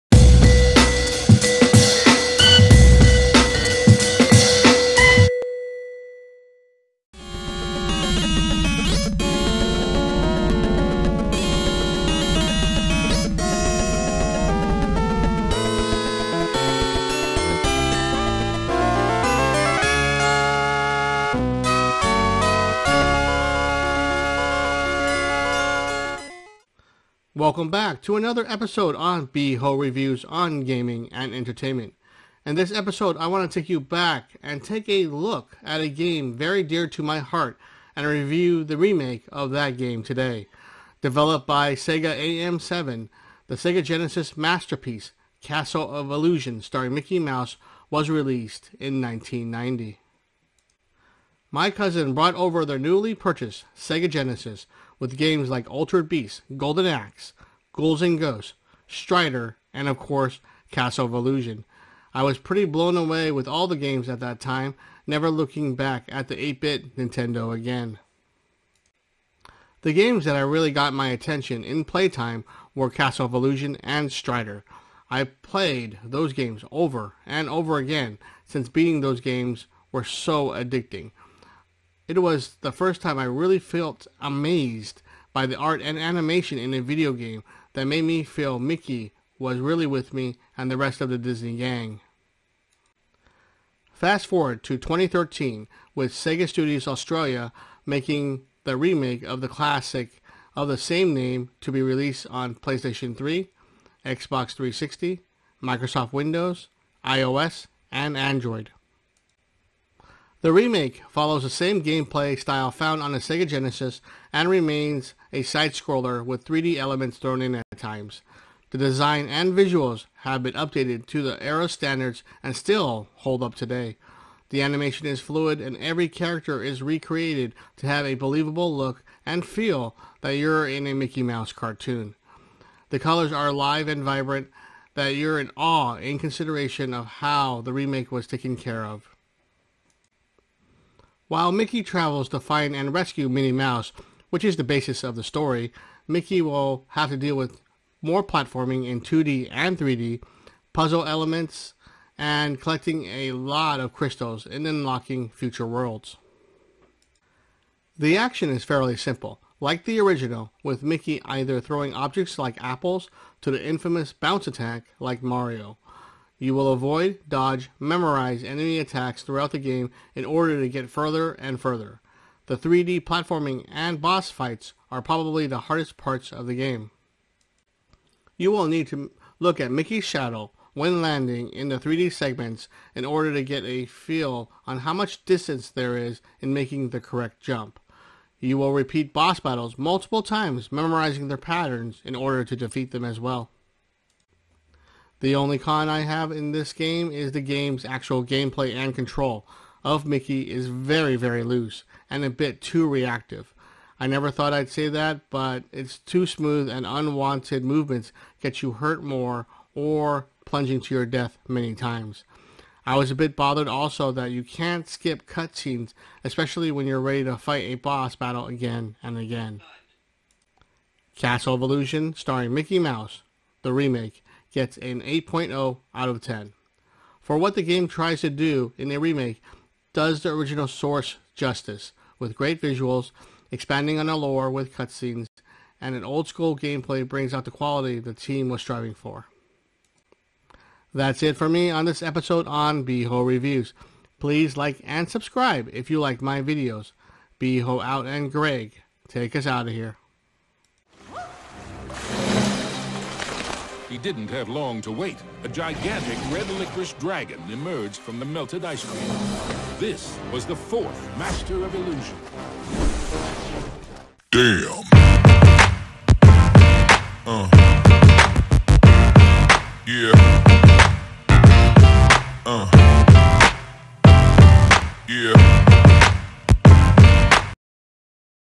CASTLE OF ILLUSION STARRING MICKEY MOUSE Review 2020
Review of Castle of Illusion Starring Mickey Mouse remake on PC (Steam), Playstation 3, and Xbox 360.

Mickey Mouse returns to star in Castle of Illusion, a fantastical reimagining of the Sega Genesis classic. When the evil witch Mizrabel kidnaps Minnie, it’s up to Mickey to brave the dangers of the Castle of Illusions to rescue Minnie. Gather your courage and traverse enchanted forests, take on hordes of rebellious toys and navigate mazes of living books. Play as Mickey and save Minnie from Mizrabel’s evil clutches!